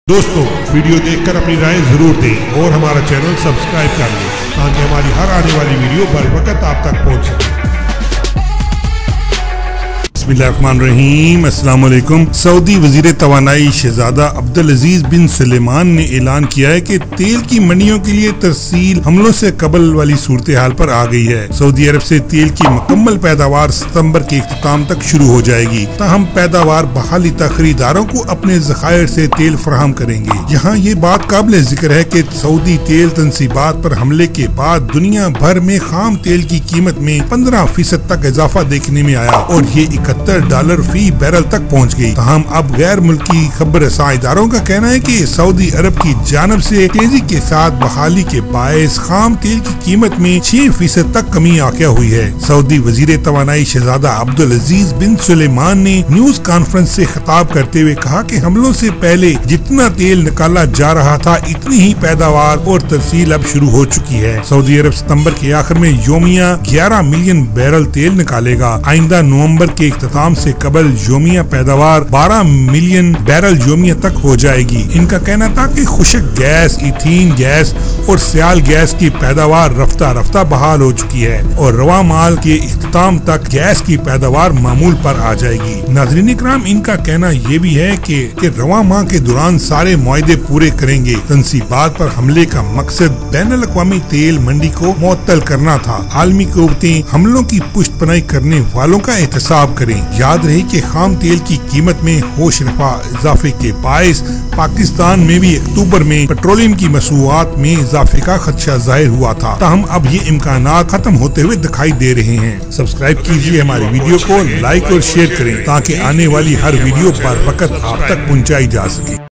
Saudi Arabia Clear Announcement about Pakistan
Saudi Arabia to restore oil output fully by end of September: energy minister Detail in Video